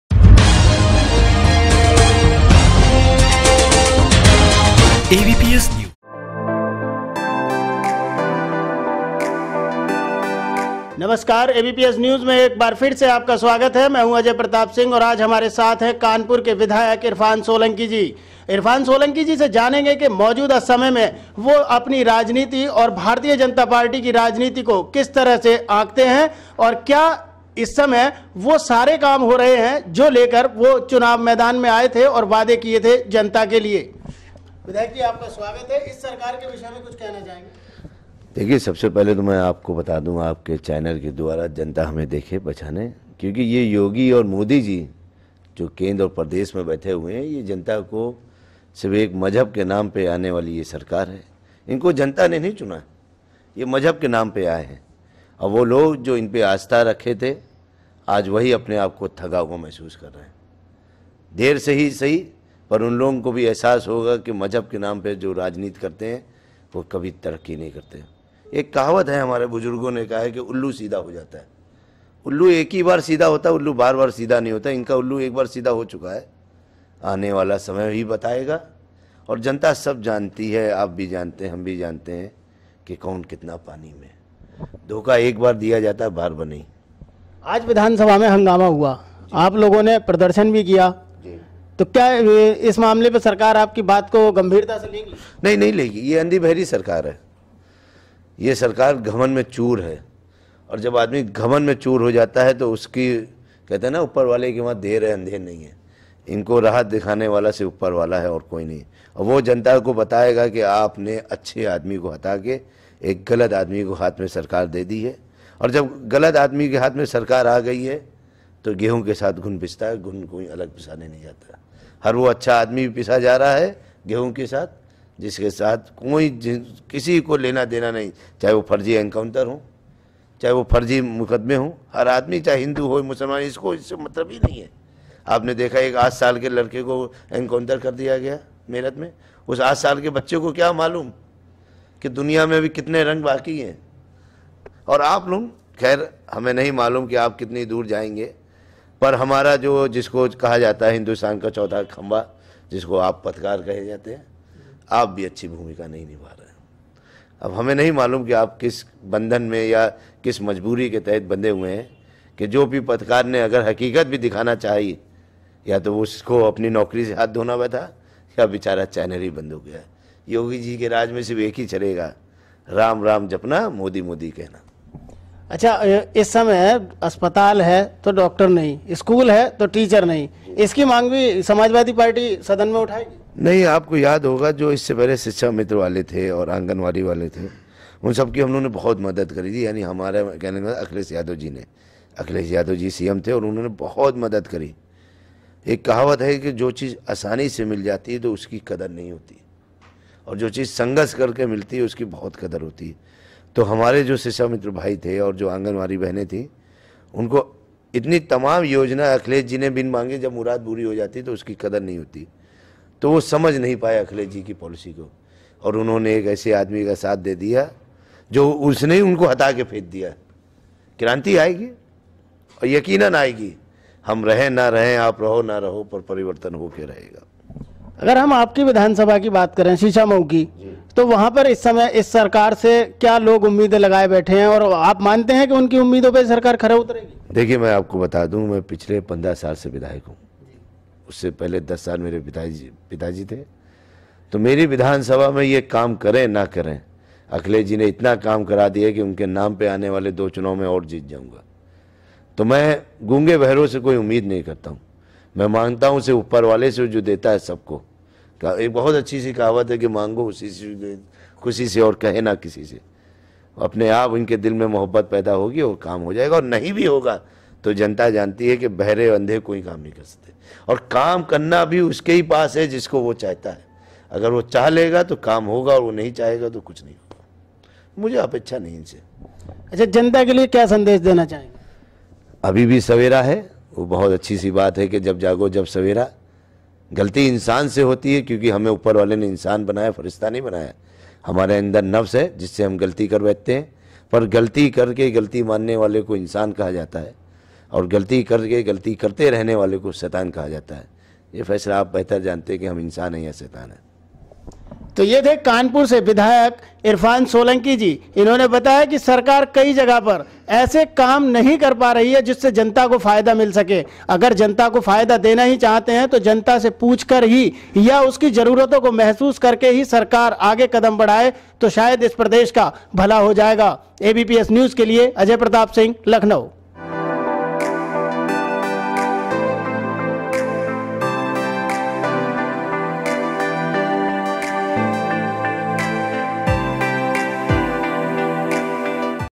मजहब की राजनीति करने वाले नहीं कर सकते तरक्की :- हाजी इरफान सोलंकी
APBS NEWS टीम के विशेष कार्यक्रम “विधायक जी चले गांव की ओर” में आज टीम से वार्ता के दौरान,  कानपुर के सपा विधायक हाजी इरफान सोलंकी ने बताया कि मजहब की राजनीति  करने वाले नहीं कर सकते कभी भी तरक्की, साथ ही उन्होंने यह भी बताया कि वर्तमान सरकार जनता की उम्मीदों पर खरी नहीं उतर पा रही है उन्होंने वर्तमान सरकार से मांग की है कि सरकार जनता की  जरूरतों को समझें।